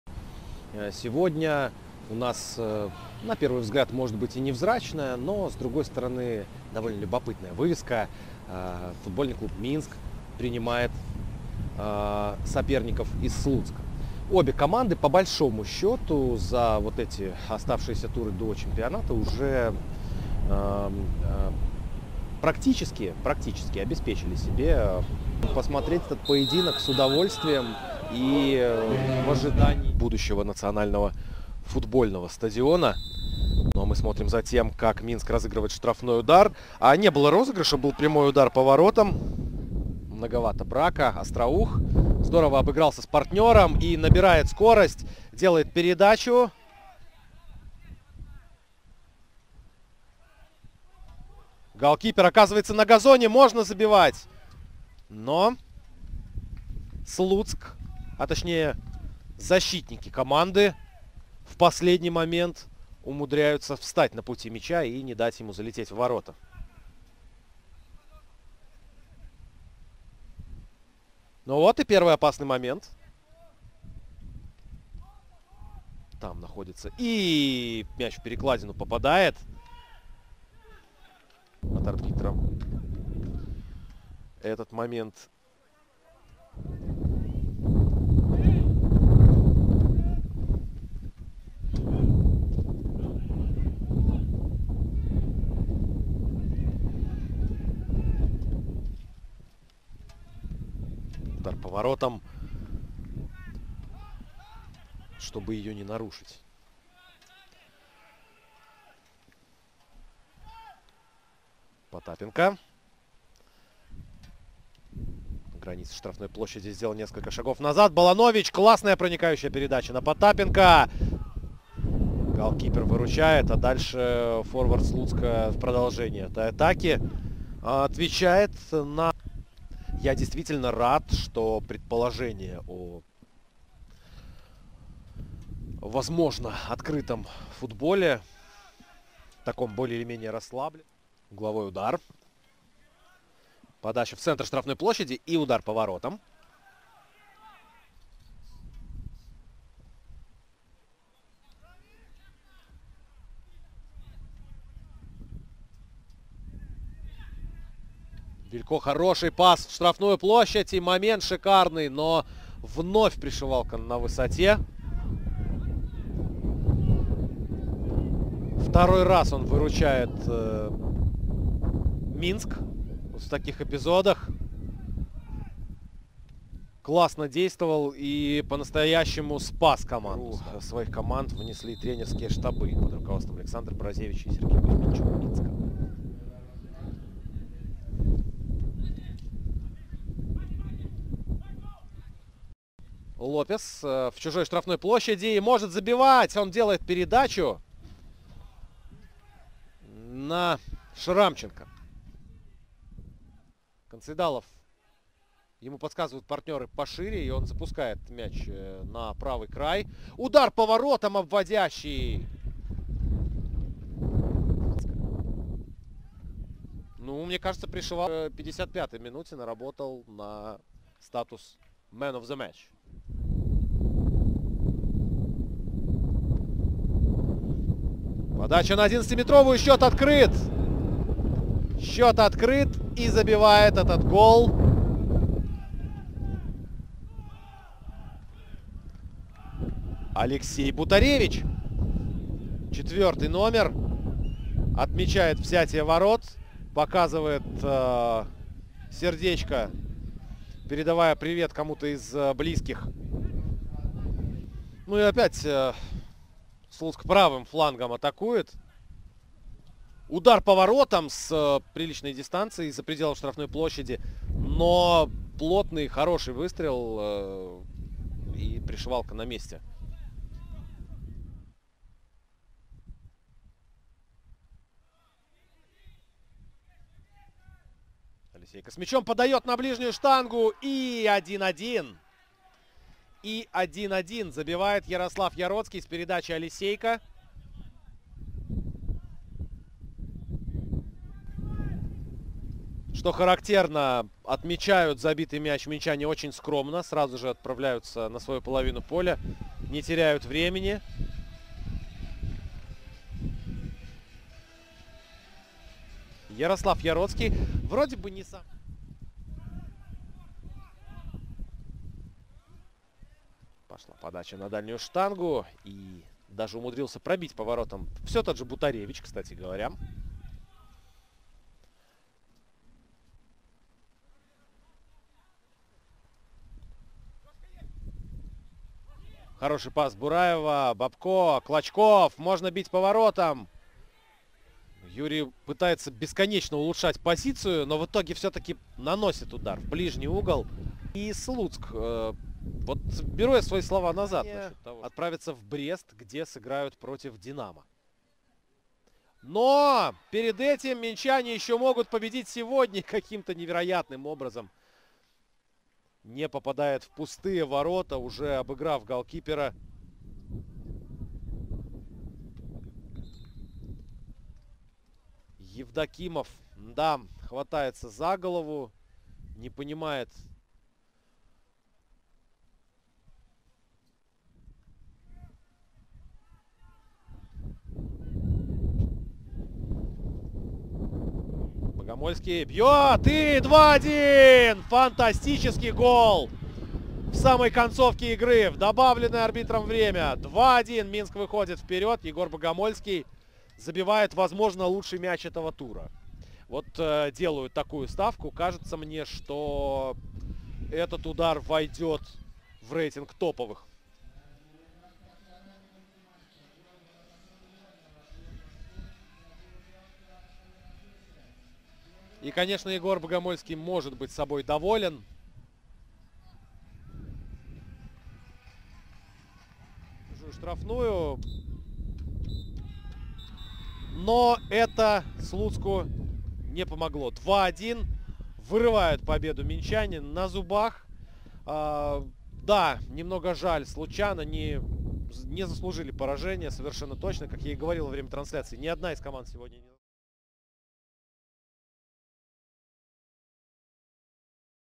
Агляд матчу Мiнск – Слуцк
Беларусбанк - Вышэйшая лiга. 24 тур. ФК Мiнск – ФК Слуцк – 2:1.
Галы: 0:1 – Бутарэвiч (56), 1:1 – Яроцкі (73), 2:1 – Багамольскі (94).

Беларусбанк-Вышэйшая ліга
Далучыся да нас у сацыяльных сетках: